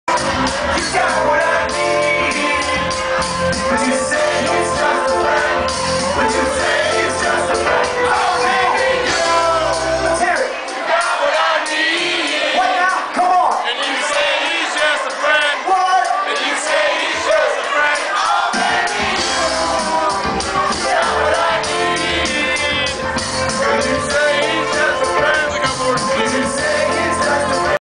Speakeasy 1st level show
these guys were great.  what a contrast and an added bonus to make the trek to check them out while taking a break from the serenity of the terrace.